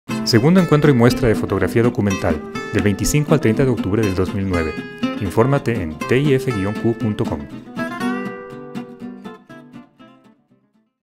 CLIP FOTODOCUMENTAL 2009 (del 25 al 30 de octubre en Quito, Ecuador)
El FOTODOCUMENTAL 2009 es un festival internacional de fotografía documental. Talleres, conferencias, conversatorios, lecturas de portafolios, encuentro con autores internacionales y muchas actividades más.
Del 25 al 30 de octubre en Quito, Ecuador